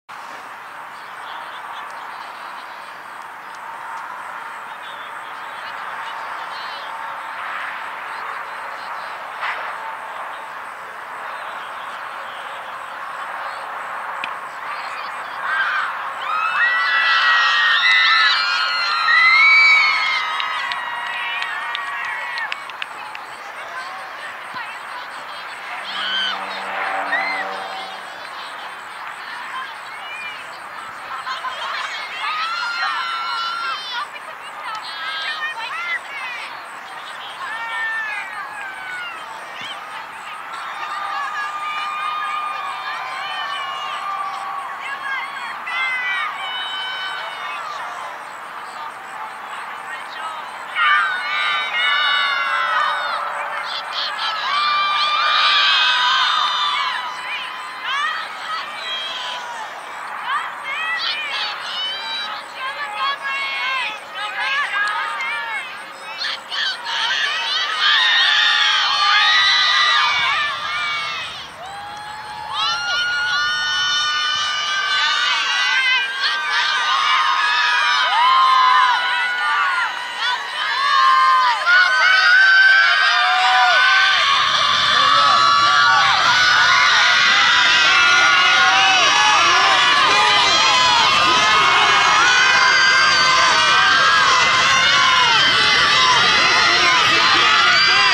Kentucky State Cross Country Champions Girls 3A 2015
Start of girls HS 3A Kentucky State Championship race at the Kentucky Horse park, Lexington, Kentucky on 11/07/2015.